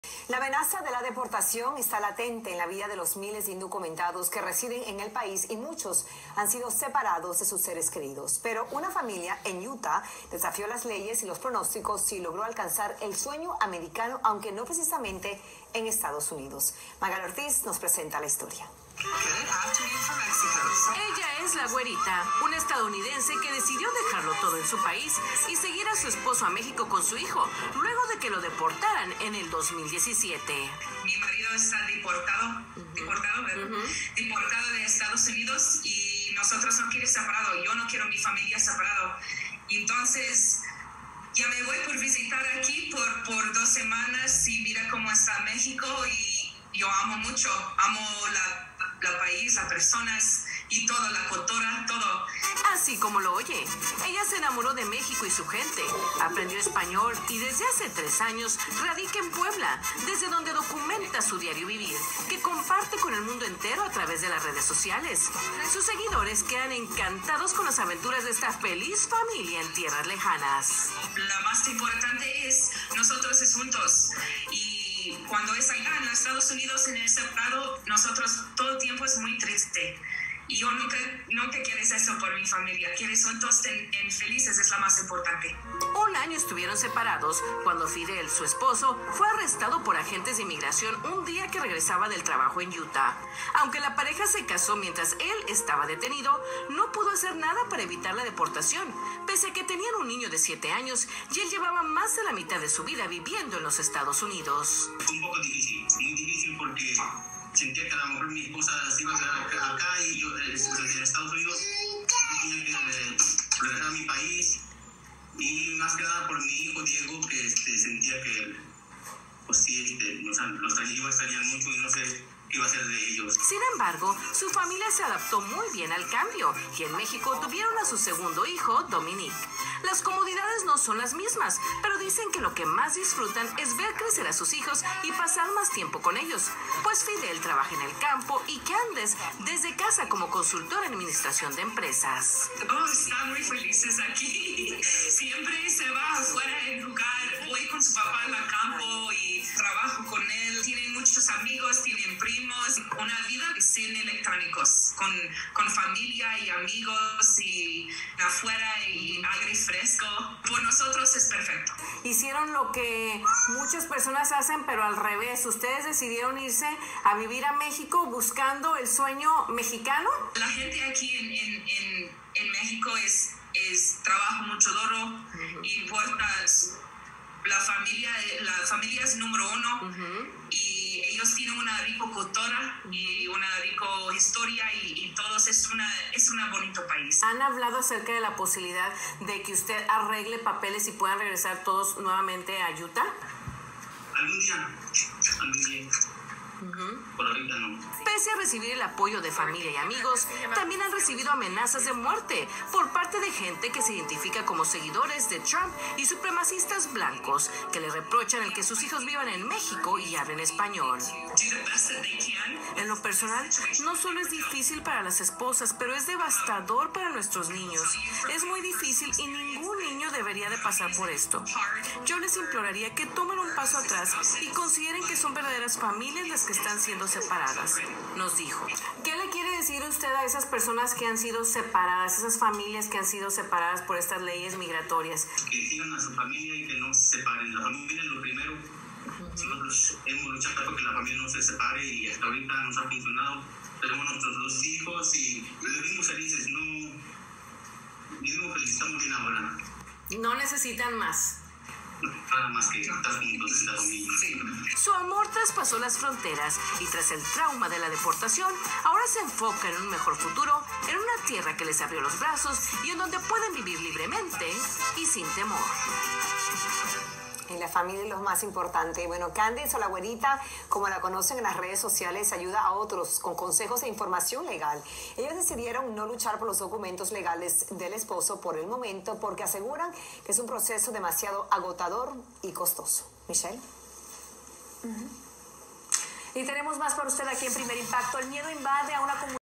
Our TV Interview! - Living in Mexico after Deportation - English Subtitles with CC on!
This channel is all about our life in Mexico after my husband's deportation. This was our first TV interview with @magalyortiztv3074 . We are so thankful for the chance to have this platform and shed light on how the immigration system affects families.